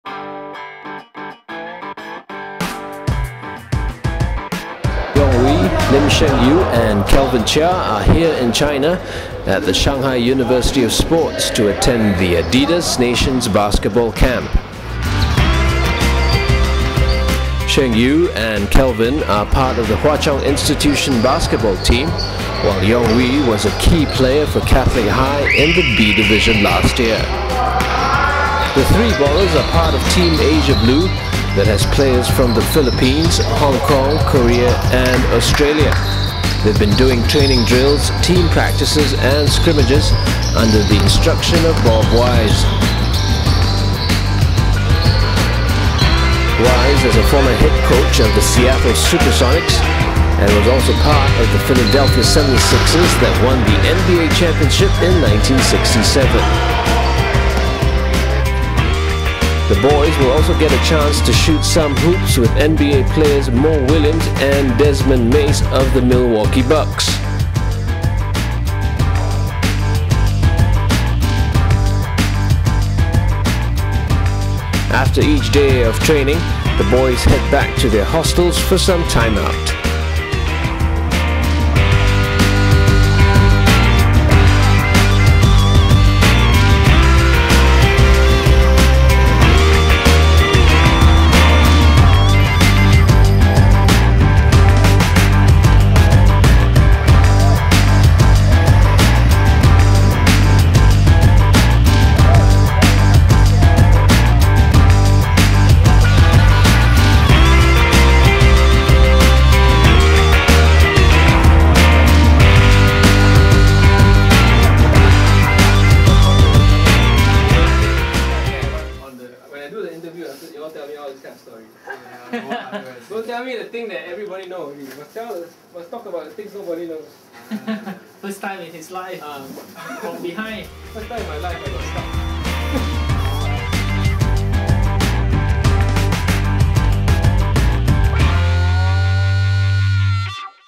adidas nations basketball camp
Khaw Yeong Wooi, Lim Sheng Yu and Calvin Chia attend an adidas basketball camp in Shanghai, China.